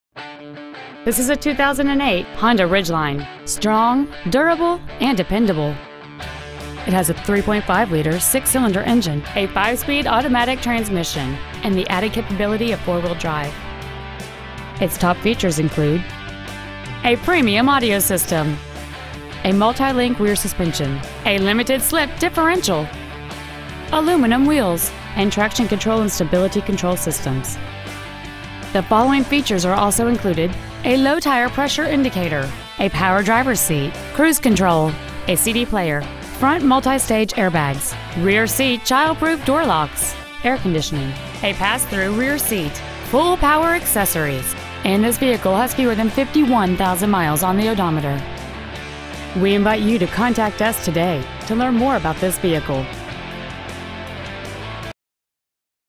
2008 Honda Ridgeline Burlington WA 98226
We've been honored to serve the Burlington WA area , we promise that your experience at our dealership will exceed your expectations ! Year : 2008 Make : Honda Model : Ridgeline Engine : 3.5L V6 Trans . : 5 Spd Automatic Exterior : Dark Cherry Pearl Miles : 50,089 Interior : Beige Sims Honda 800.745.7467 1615 S. Goldenrod Road Burlington , WA 98233 Preowned 2008 Honda Ridgeline Burlington WA